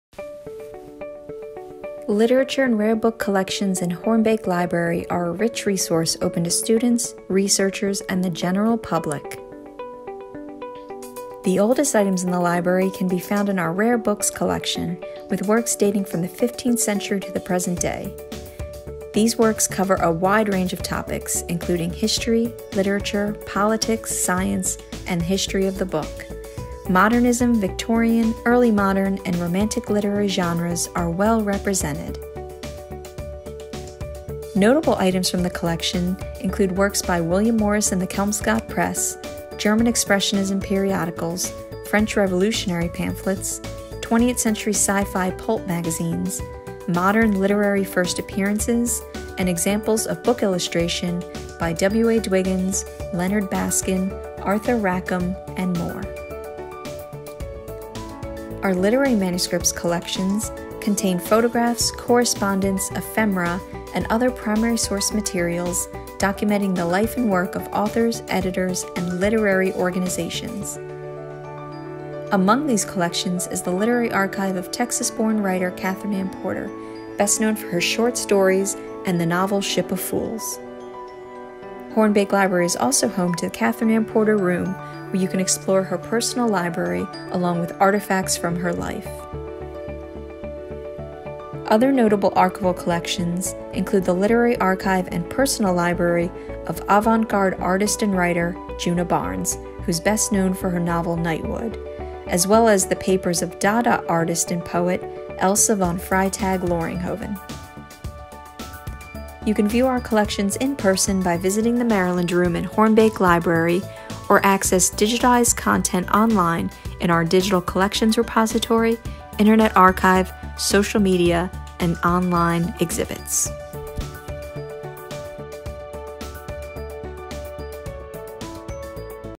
UMD's Literature and Rare Books in the Special Collections and University Archives
Our collections include book and manuscript holdings that document the lives and work of significant literary figures, encompassing artists, editors, journalists, poets, and writers as well as books, pamphlets, and ephemera dating from the 15th century to the present day spanning multiple languages, with subjects encompassing the humanities.